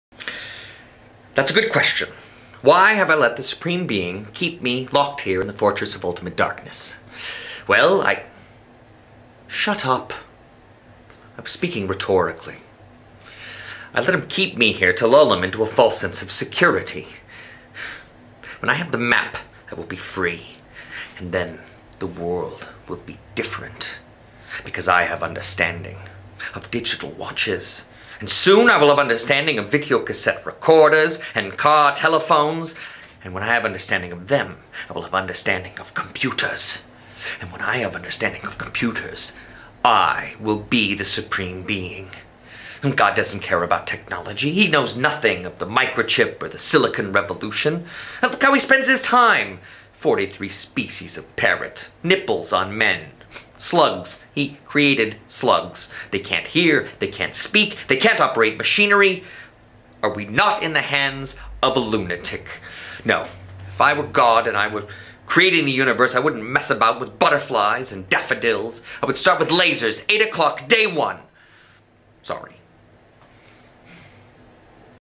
Monologue a Day - Keith Coogan - Day 137
Monologue a Day project... Keith Coogan learns and performs a monologue a day as inspired by the book and film, 'Julie and Julia'. Shot on the Sony HDR-HC1 in Los Angeles, CA. Cut with iMovie on an iMac. Day 137 is from "Time Bandits" written by Terry Gilliam and Michael Palin.  Evil expounds on his plans once he gets his hands on the map that was stolen by time bandits.

Day 137: "Time Bandits" - Terry Gilliam and Michael Palin - Film